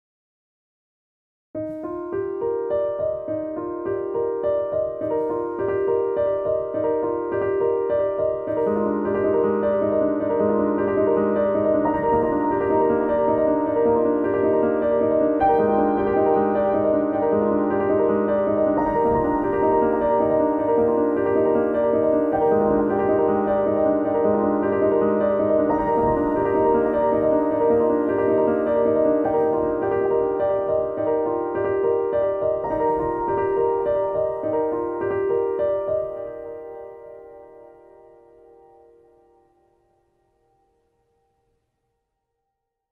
Snowflake
Season’s Greetings from Helsinki Metropolia University of Applied Sciences. Animated greetings by our Graphic Design students.